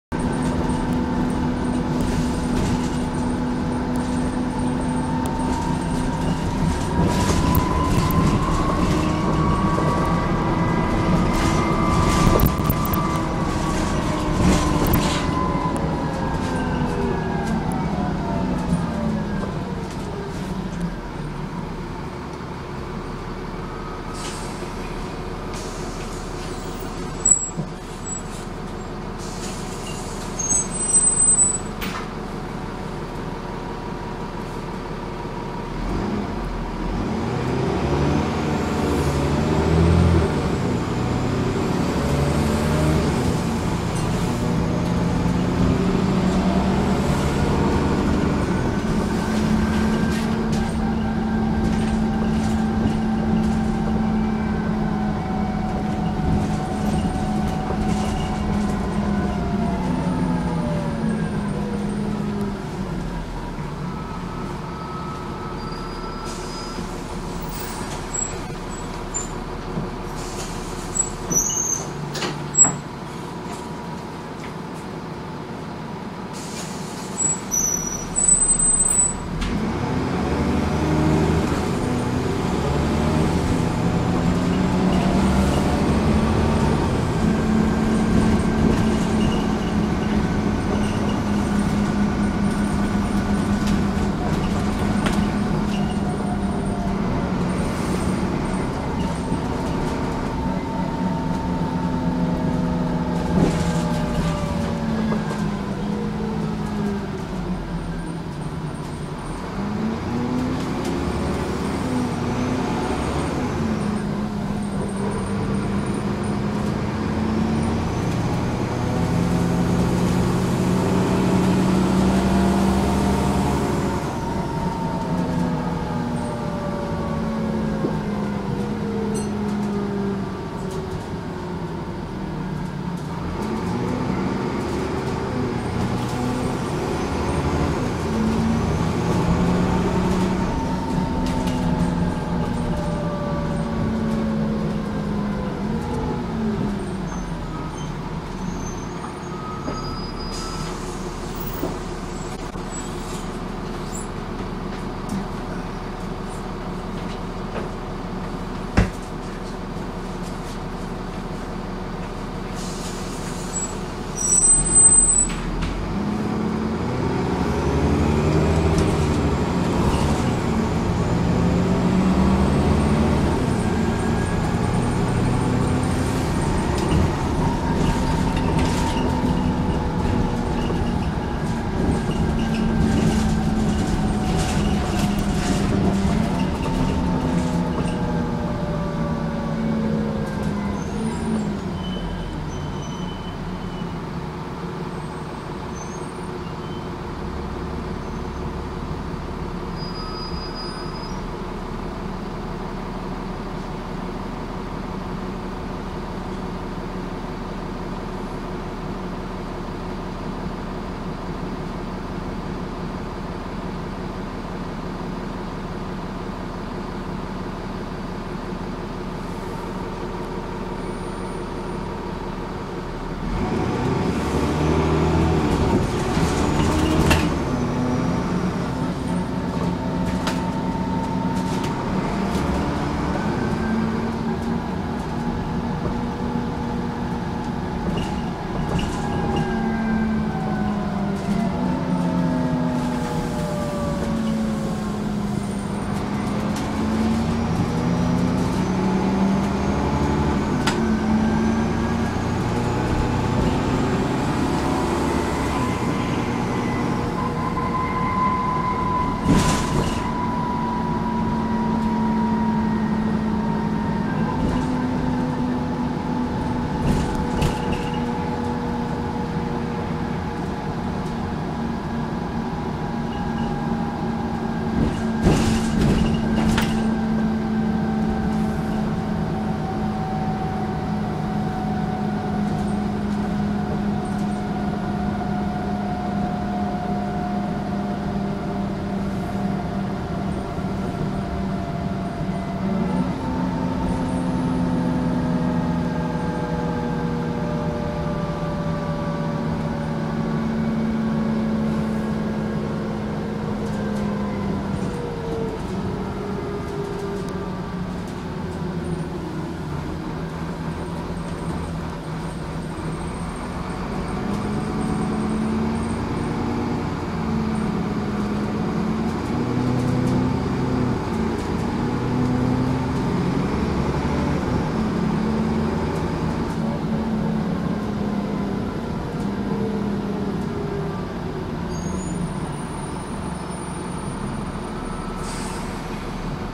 Stagecoach Devon ML02RWO
Stagecoach 17701 is seen on route 12A.